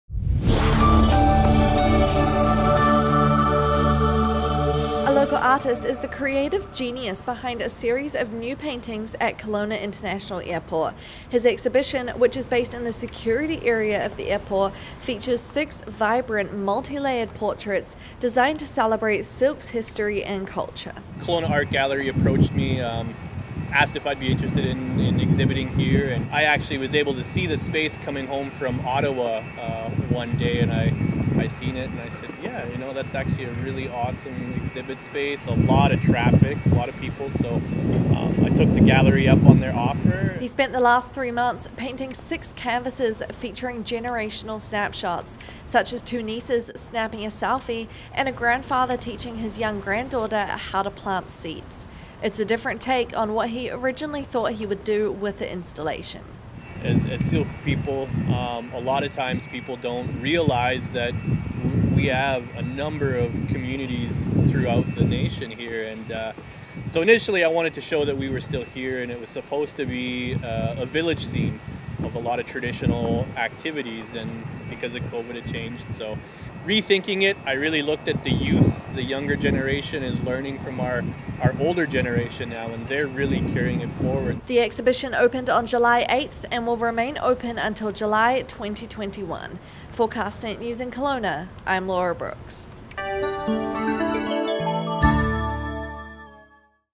Local artist unveils new syilx exhibition at airport
Sheldon Pierre Louis partners with Kelowna Art Gallery for latest installation.